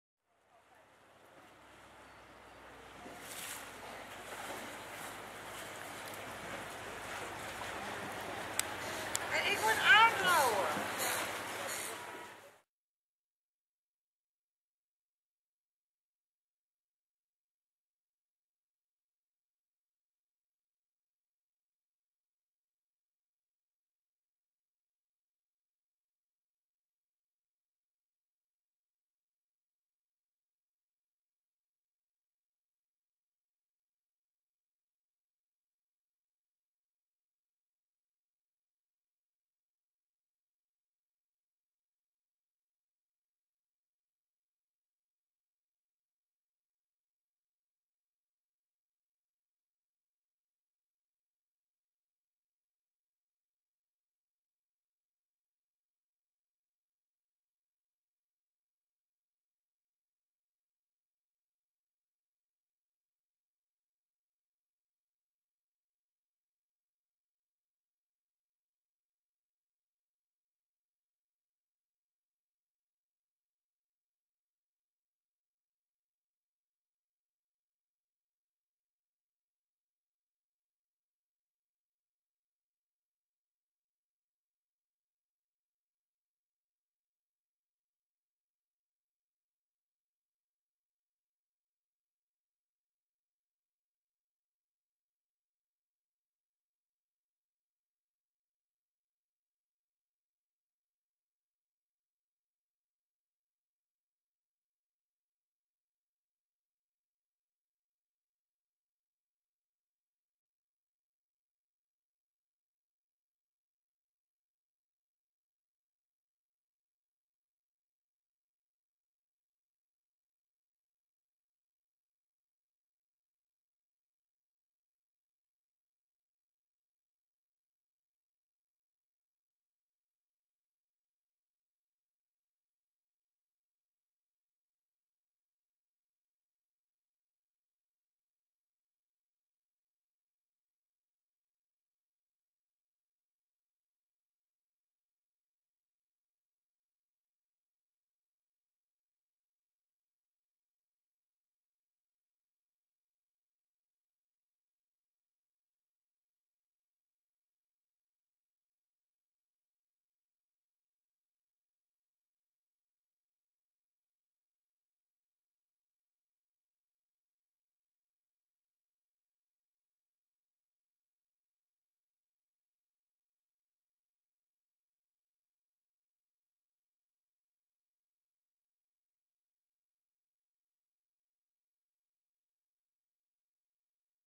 Mariposa - From Schiedam to Amazonehaven
Timne laps clip from a boat trip on a sunny afternoon with the barge Mariposa, passing the Breediep